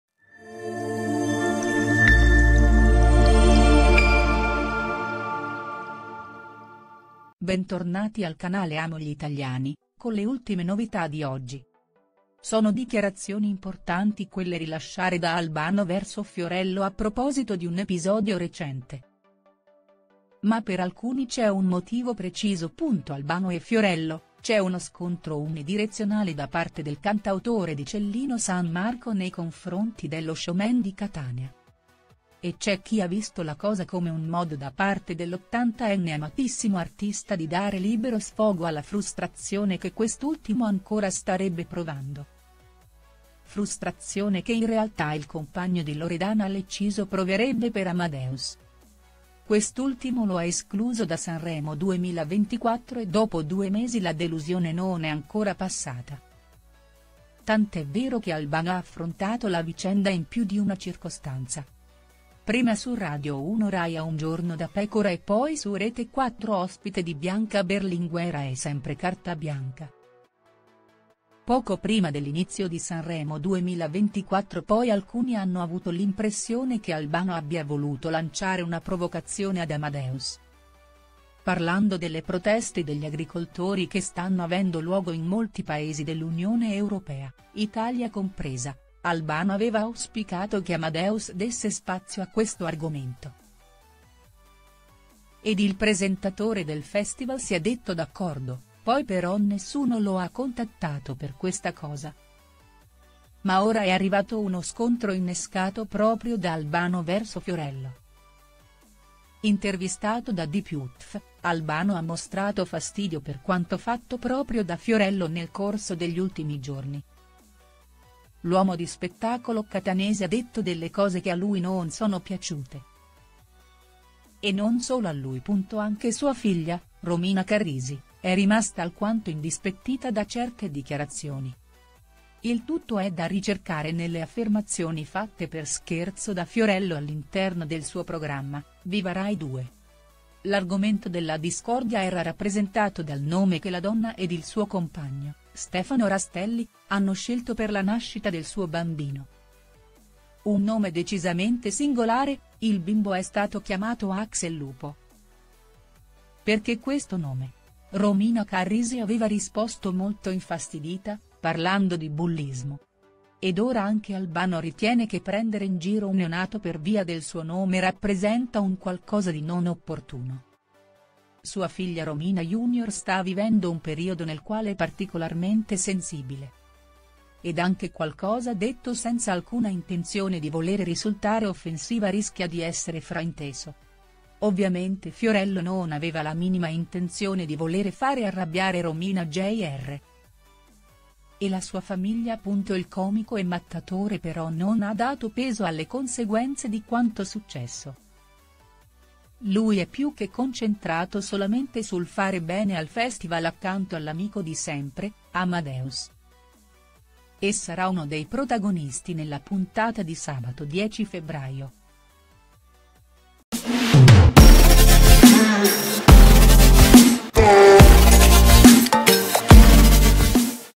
Al Bano attacca Fiorello, parole durissime del pugliese contro il presentatore siciliano
Al Bano e Fiorello, c’è uno scontro unidirezionale da parte del cantautore di Cellino San Marco nei confronti dello showman di Catania. E c’è chi ha visto la cosa come un modo da parte dell’80enne amatissimo artista di dare libero sfogo alla frustrazione che quest’ultimo ancora starebbe provando.

Frustrazione che in realtà il compagno di Loredana Lecciso proverebbe per Amadeus. Quest’ultimo lo ha escluso da Sanremo 2024 e dopo due mesi la delusione non è ancora passata . Tant’è vero che Al Bano ha affrontato la vicenda in più di una circostanza.

Prima su Radio 1 Rai a “Un giorno da pecora” e poi su Rete 4 ospite di Bianca Berlinguer a “È sempre CartaBianca”. Poco prima dell’inizio di Sanremo 2024 poi alcuni hanno avuto l’impressione che Al Bano abbia voluto lanciare una provocazione ad Amadeus.